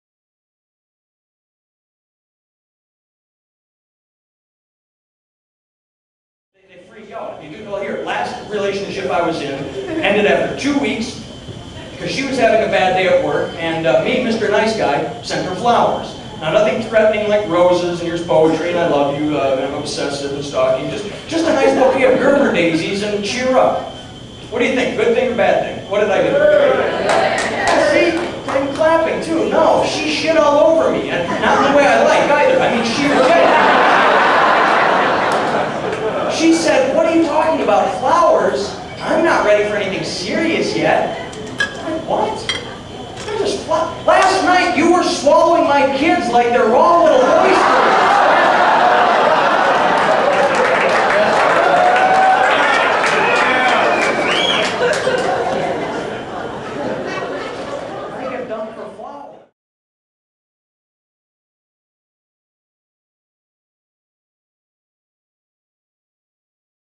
nathan timmel: a single fella suffers
recorded at the funny bone in fairview heights, il, idiot comedian nathan timmel whines about what it's like to be single.